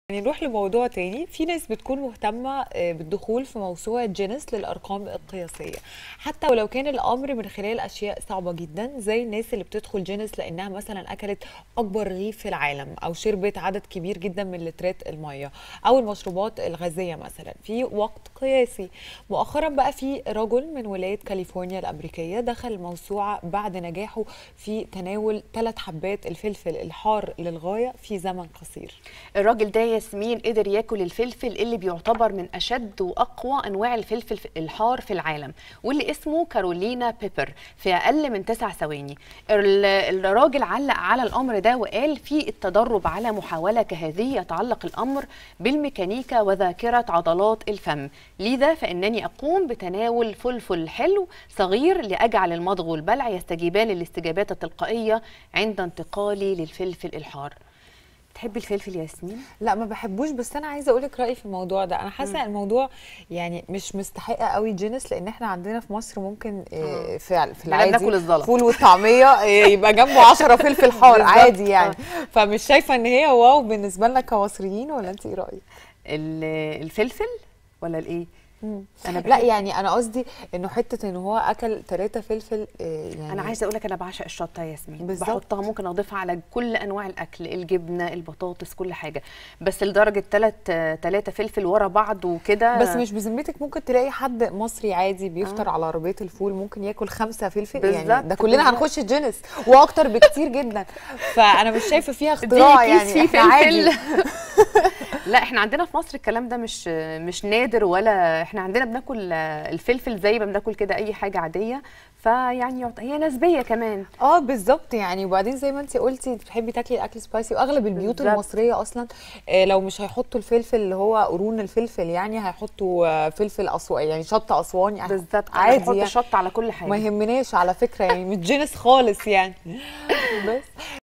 أمريكي يحطم الرقم القياسي بأكل الفلفل الأكثر حرا في العالم ويدخل "جينيس"
في ناس بتكون مهتمة الدخول في موسوعة جينيس للأرقام القياسية حتى ولو كان الأمر من خلال أشياء صعبة، زي الناس اللي بتدخل جينيش لأنها أكلت أكبر رغيف في العالم، أو شربت عدد كبير من لترات المياه أو المشروبات الغازية في وقت قياسي.. مؤخرًا في رجل من ولاية كاليفورنيا الأميركية دخل الموسوعة بعد نجاحه في تناول 3 حبات الفلفل الحار للغاية في زمن قصير.